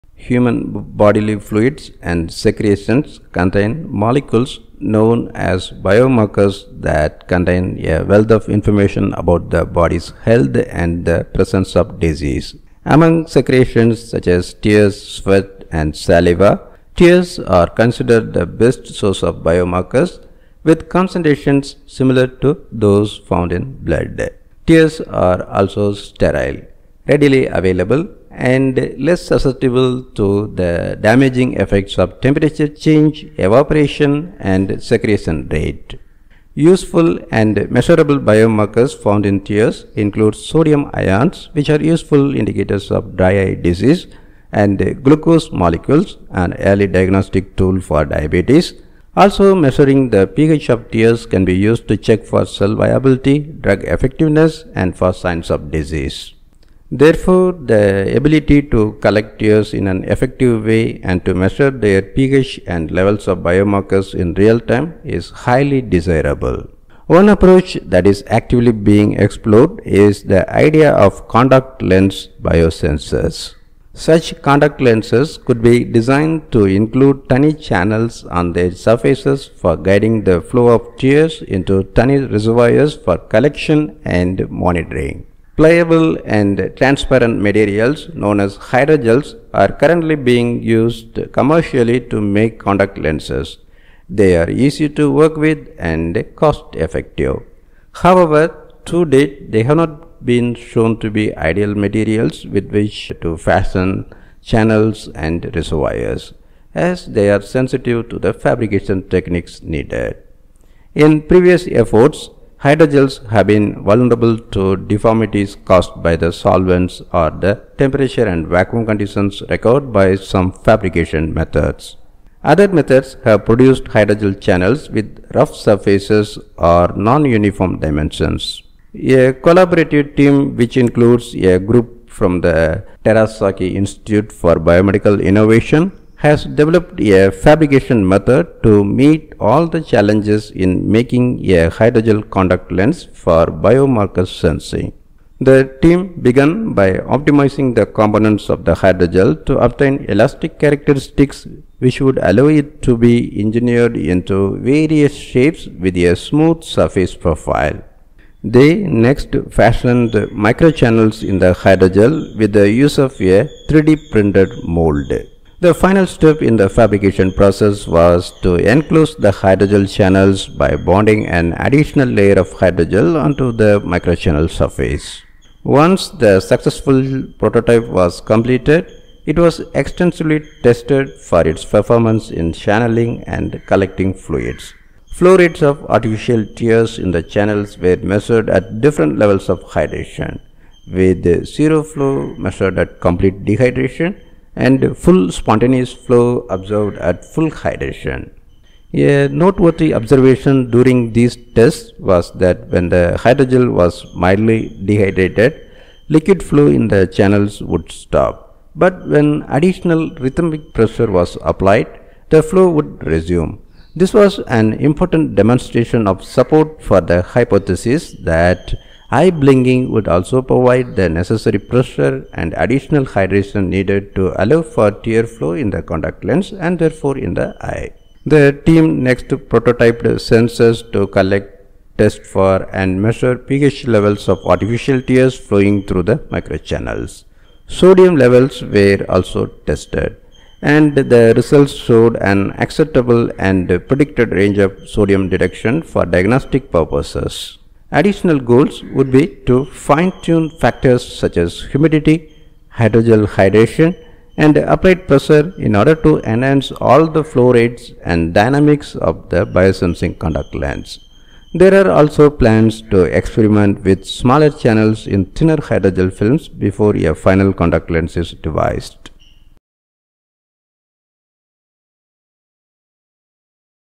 Contact lenses for diagnostic and therapeutic use
Specially engineered contact lenses use tears to monitor patient health .

A collaborative team, which includes a group from the Terasaki Institute for Biomedical Innovation, has developed a fabrication method to meet all the challenges in making a hydrogel contact lens for biomarker sensing.  The team began by optimizing the components of the hydrogel to obtain elastic characteristics which would allow it to be engineered into various shapes with a smooth surface profile.  They next fashioned microchannels in the hydrogel with the use of a 3D printed mold.  The final step in the fabrication process was to enclose the hydrogel channels by bonding an additional layer of hydrogel onto the microchannel surface.

Credit: Khademhosseini Lab